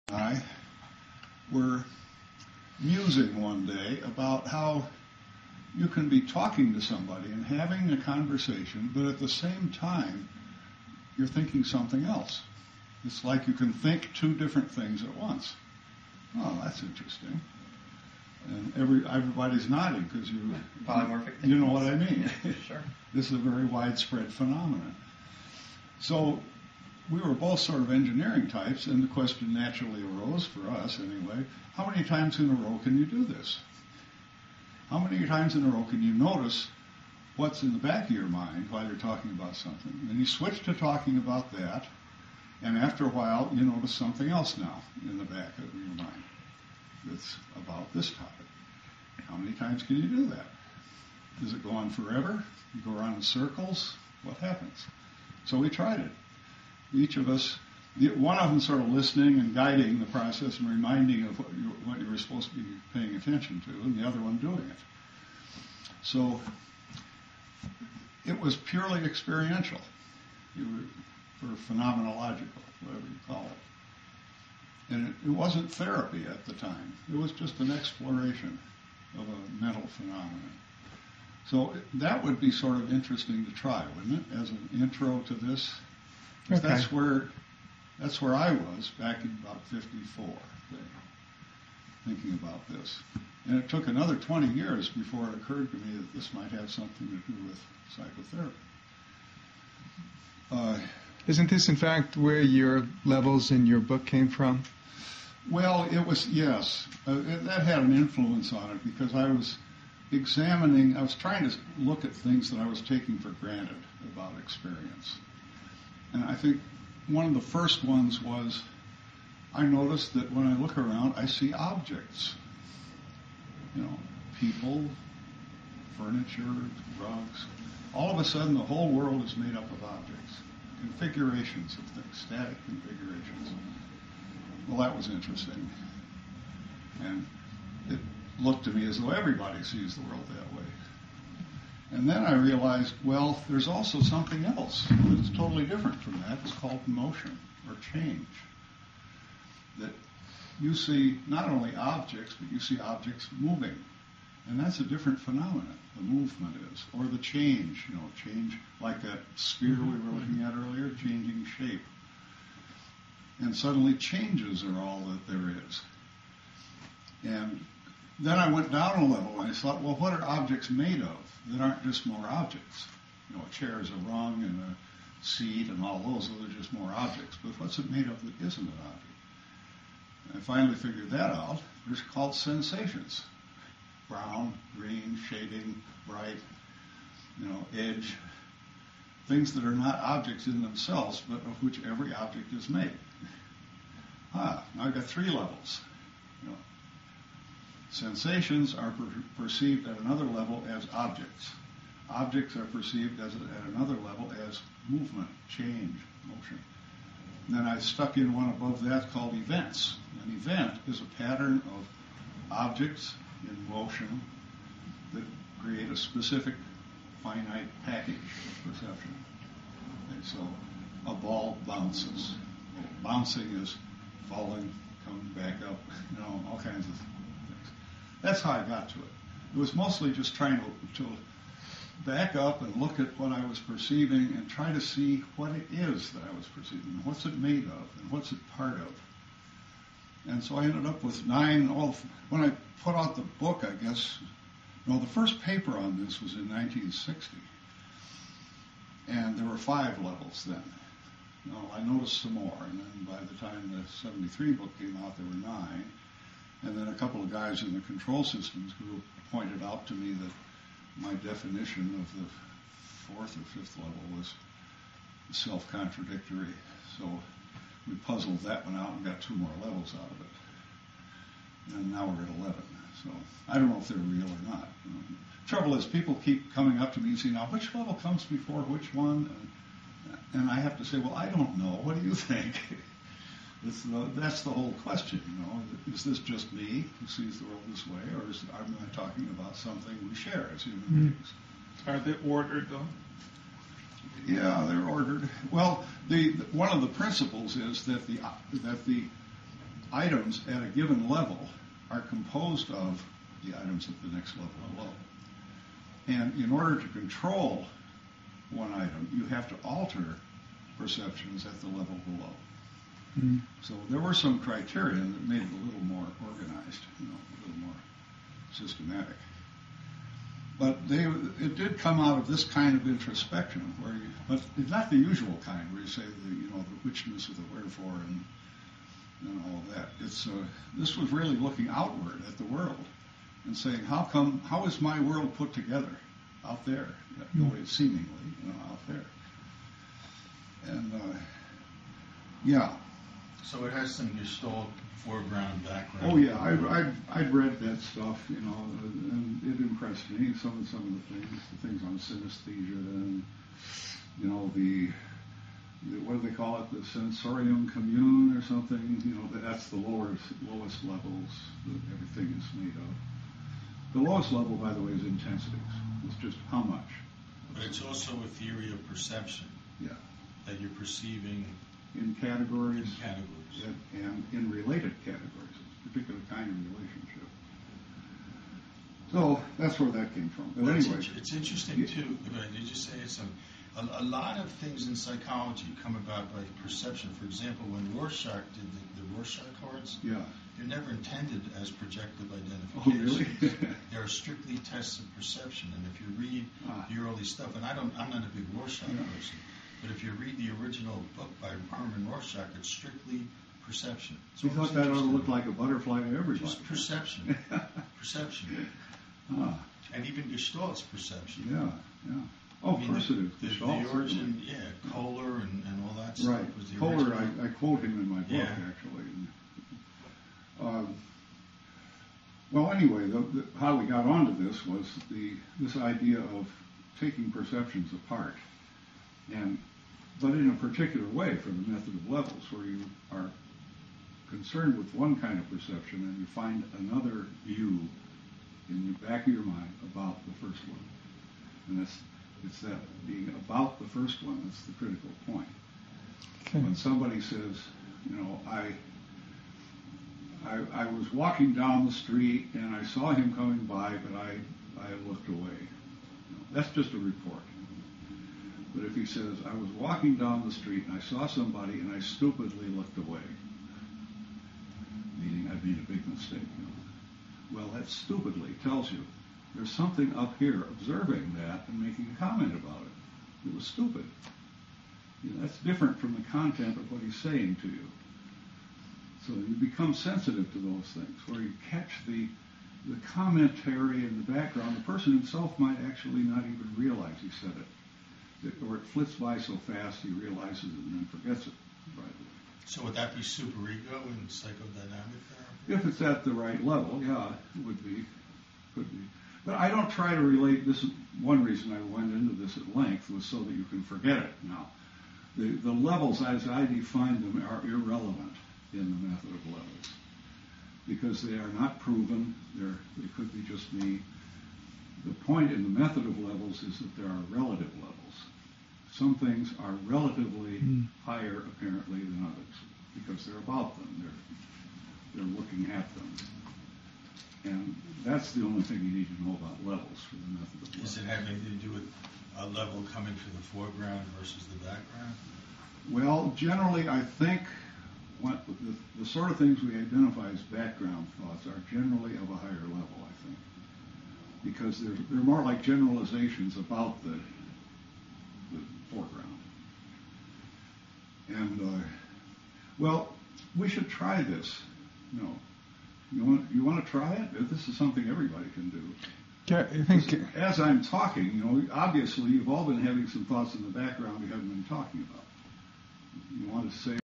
The origins of MOL Therapy
William T. Powers describes the origins of MOL Therapy at the Edgemoor Conference in Cherry Hill, NJ. The people in attendance are Drs. Gary Padover, John White, Carter Cloyd, David London and David Goldstein who is the camera person.